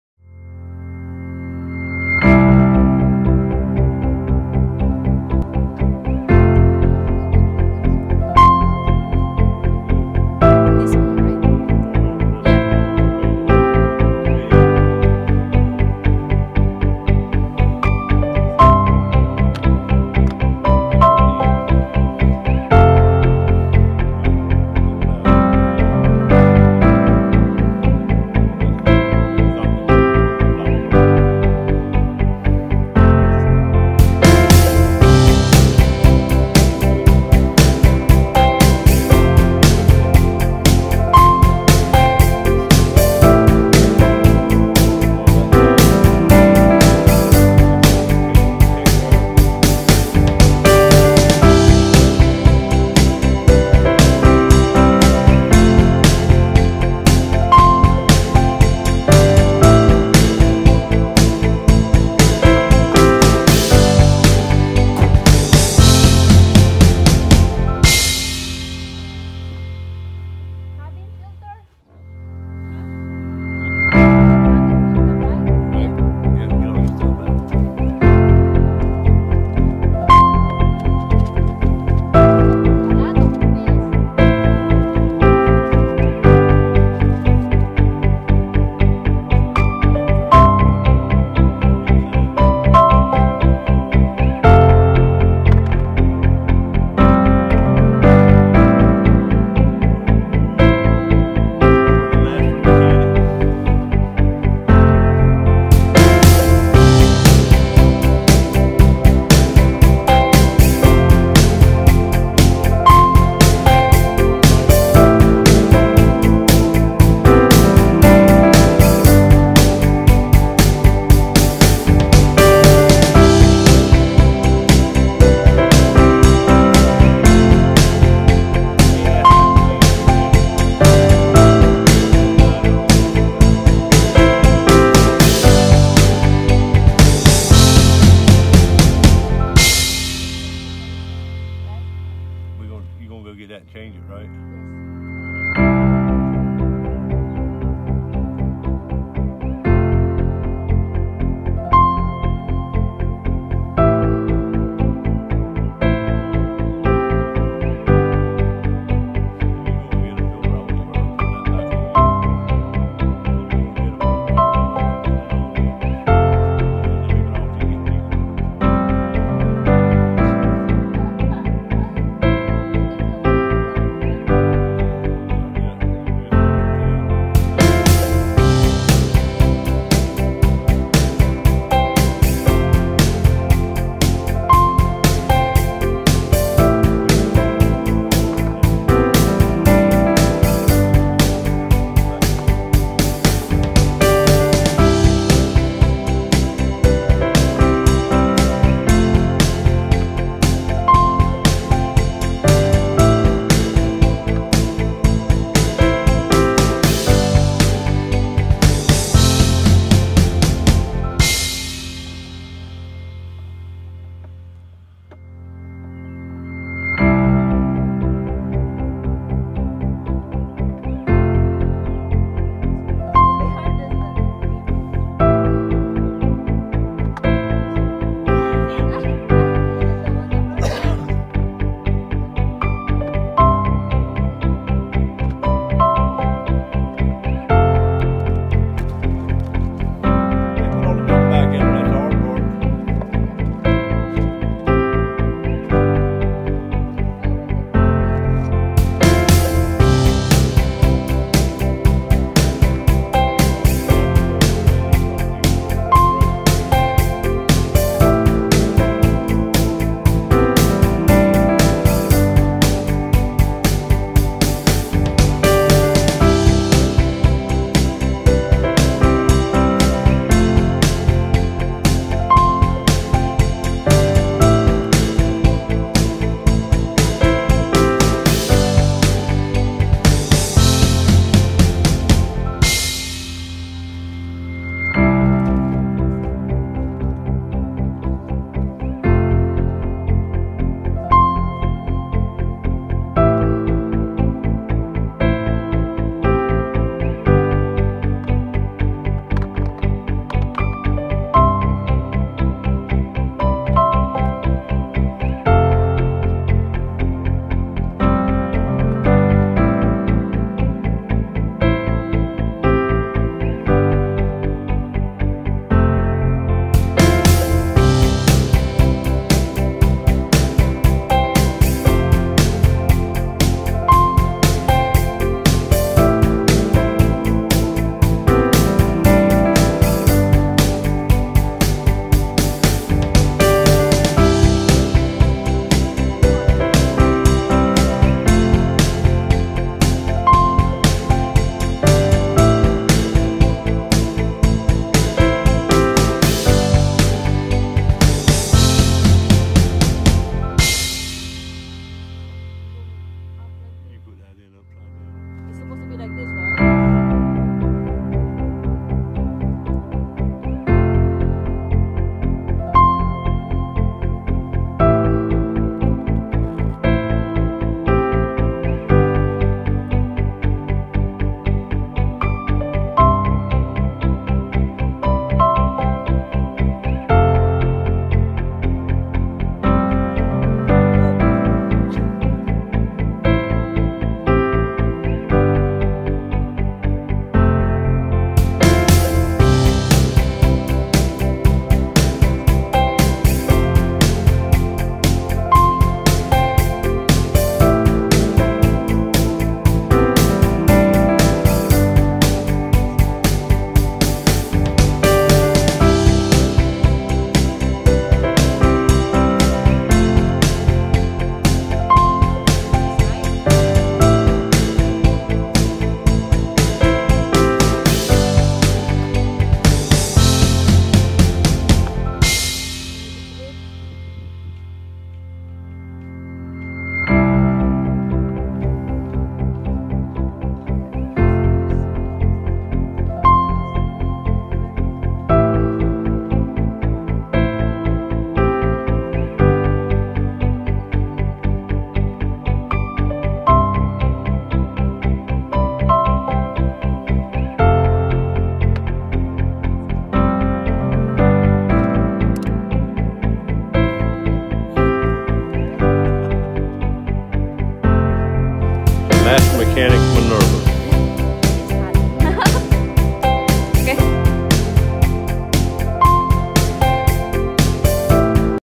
First time checking car filter and changing cabin filter
Excited to checking car filter and changing cabin filter|filipina live in NC